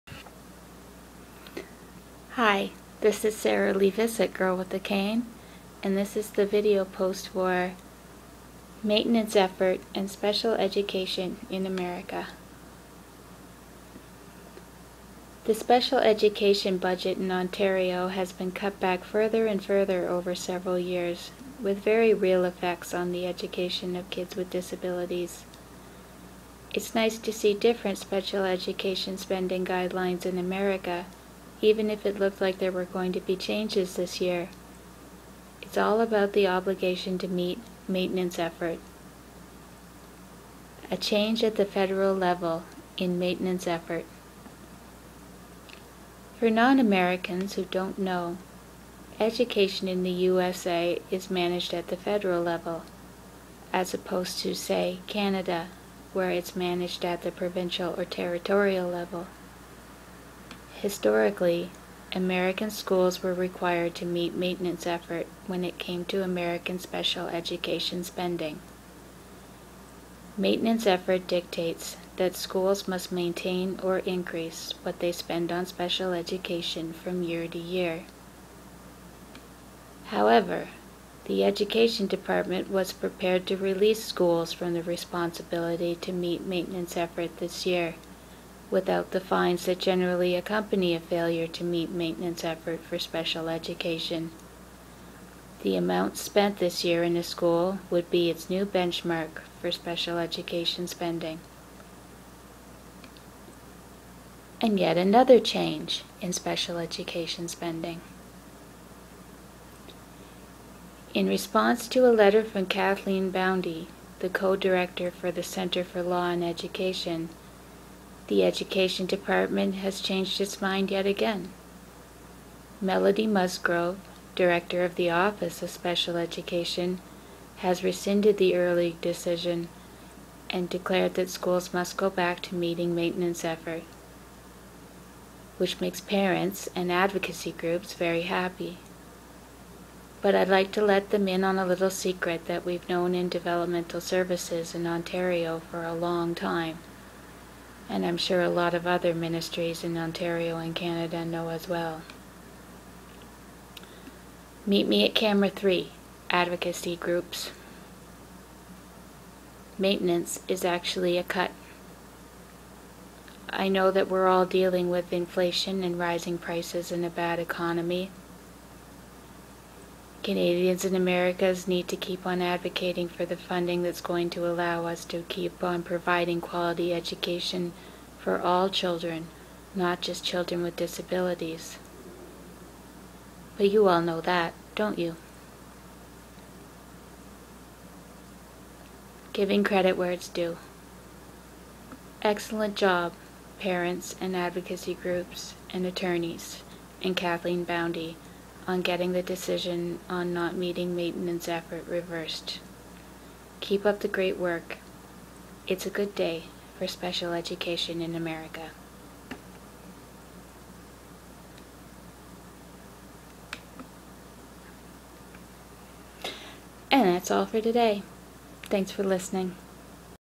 Maintenance Effort.wmv
Sarah Levis reads a post from her award-winning blog, "Girl With The Cane". For more information about Sarah and "Girl With The Cane",